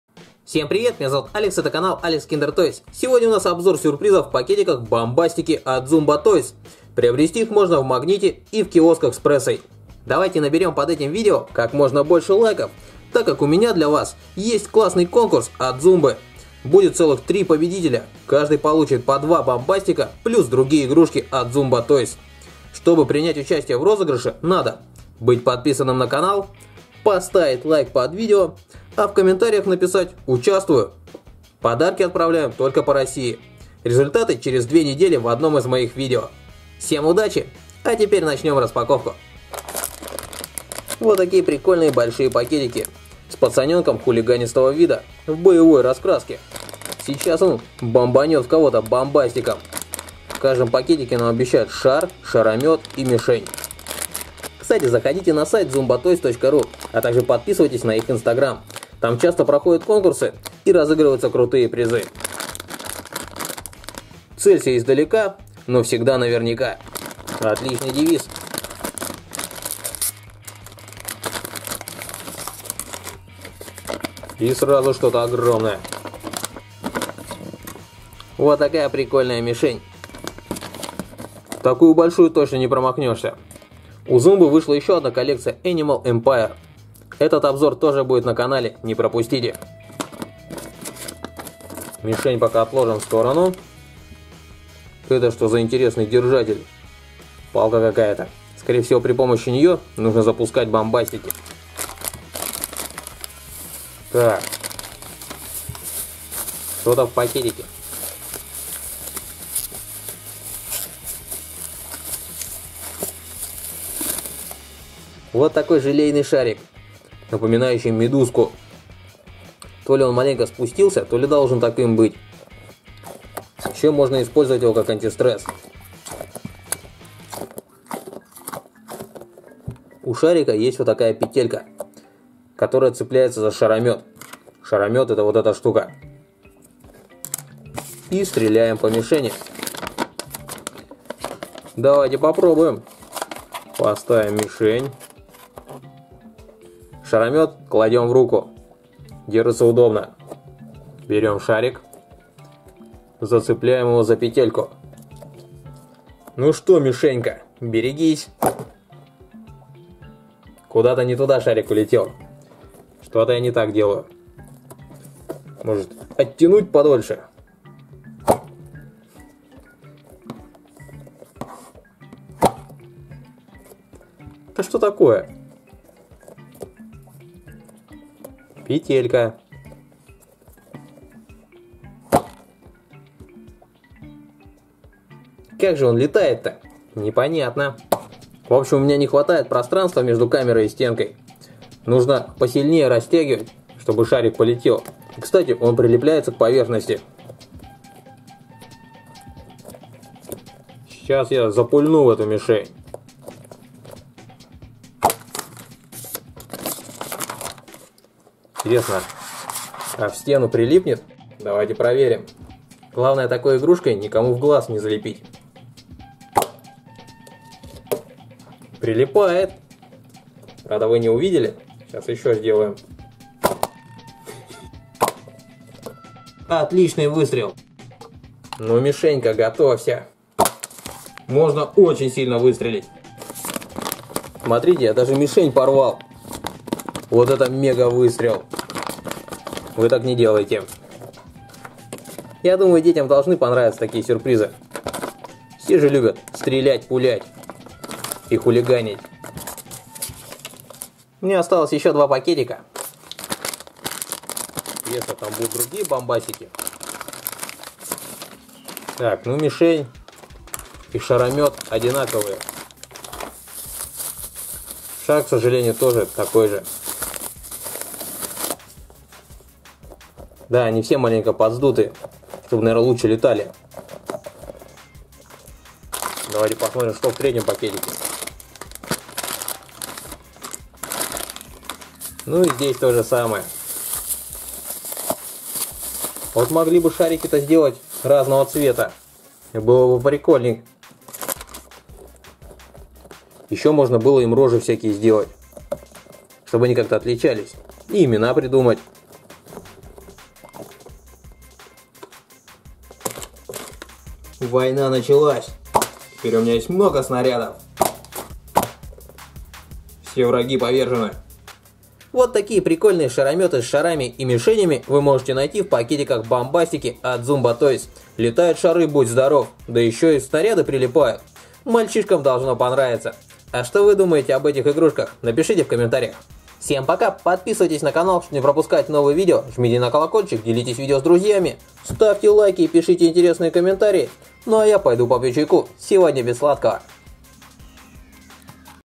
БОМБАСТИКИ от ZOOMBA! Обзор Сюрпризы в пакетиках, Игрушки для детей + КОНКУРС Surprise unboxing
Бомбастиков можно купить в магазинах Магнит и в киосках с прессой!